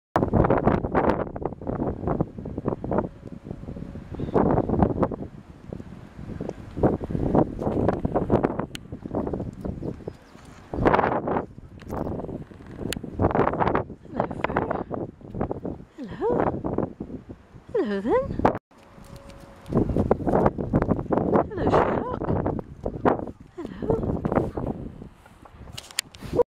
Fir & Sherlock came to see me today. Sherlock licked my camera lens!
Fir & Sherlock came along to see me today. Sherlock came so close that his tongue licked my camera lens!